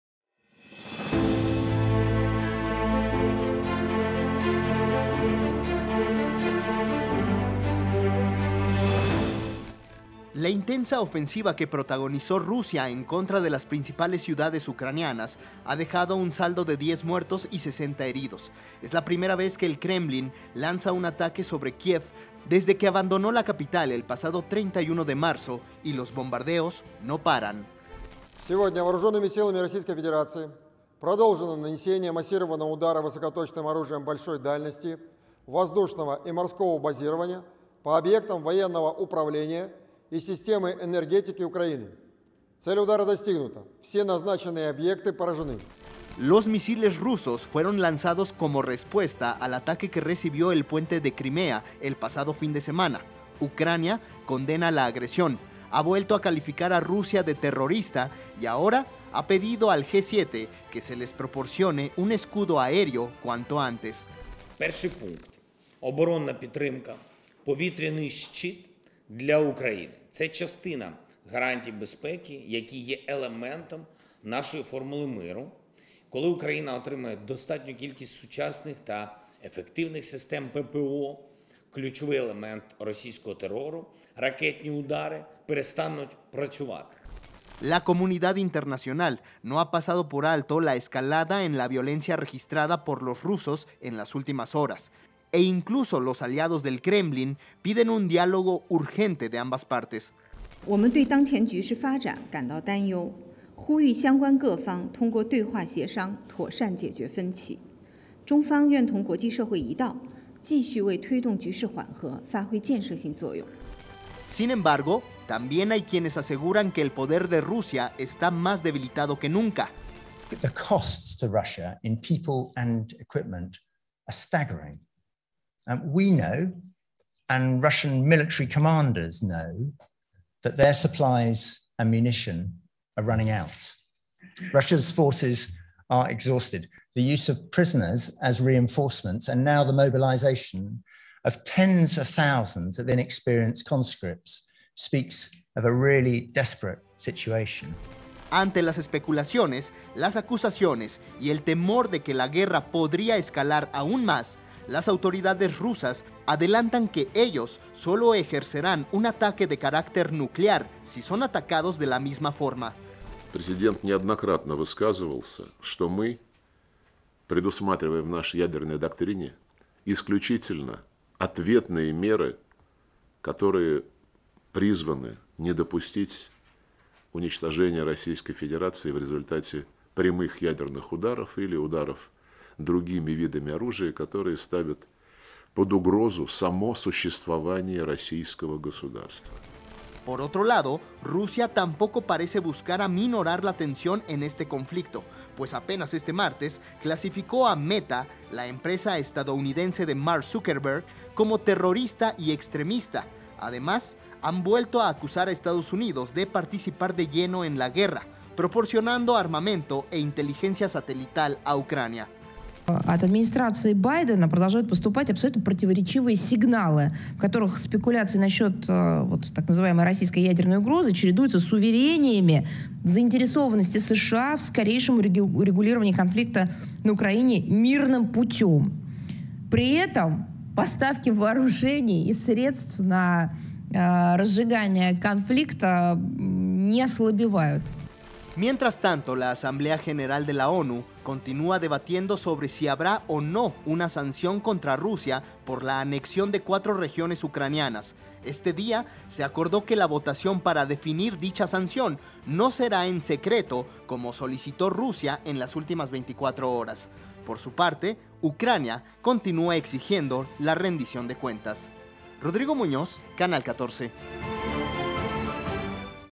Tensiones entre Rusia y Ucrania no cesan
Las tensiones entre Rusia y Ucrania no paran de crecer. Mientras ambos países permanecen en guerra, la ONU debate si sancionar tomará sanciones en contra de a Rusia por la anexión de cuatro regiones ucranianas.